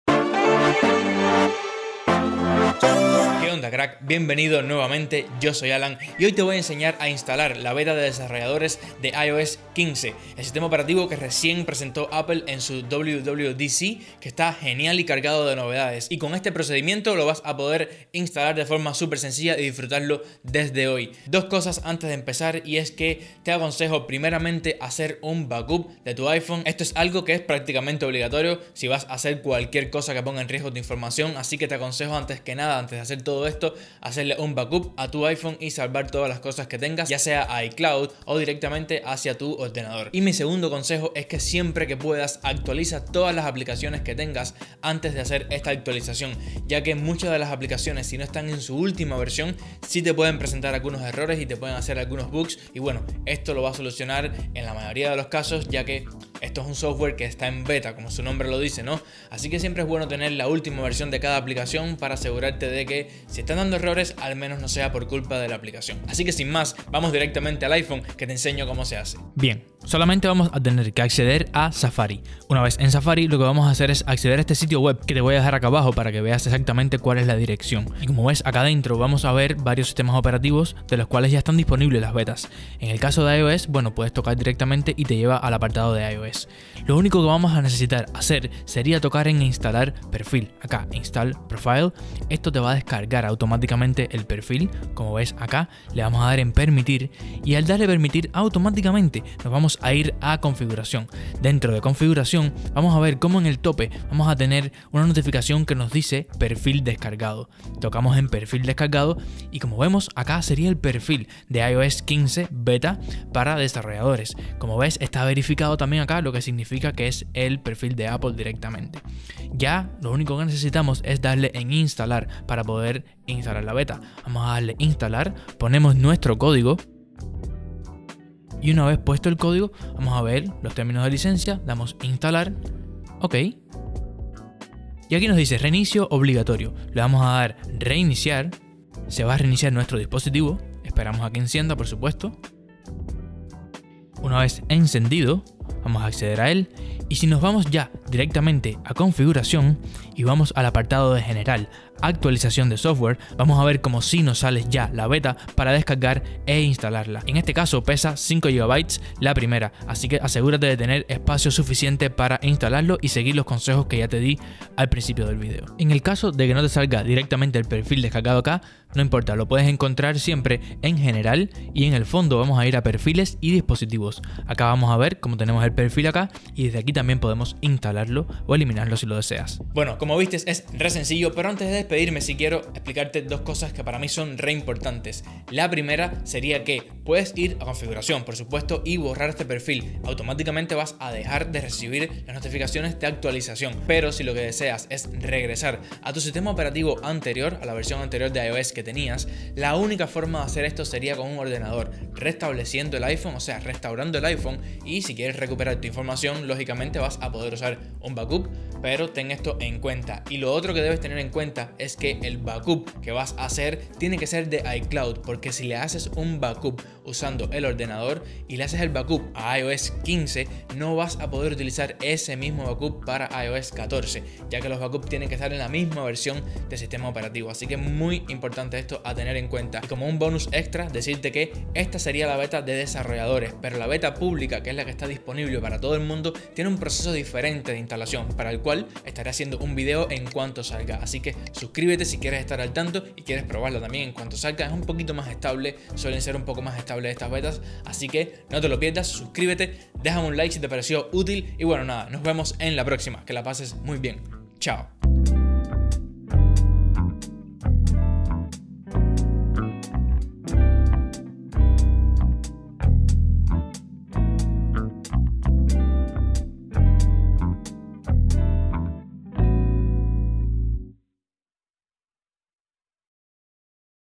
Como Instalar Beta de iOS 15, Actualizado Beta 3
Te enseño como Inslalar iOS15 Beta 3 de Desarrolladores de sin necesidad de una cuenta de manera súper sencilla y te doy mis consejospersonales para que te prepares antes de hacerlo.
Comunidad en TELEGRAM 🚀
Capítulos:
0:00 Bienvenido
0:21 Consejos antes de Instalar Betas
1:06 Como Descargar el Perfil
2:59 Como eliminar Perfil y Regresar a iOS 14
3:53 Betas Públicas
🔊Música usada en el video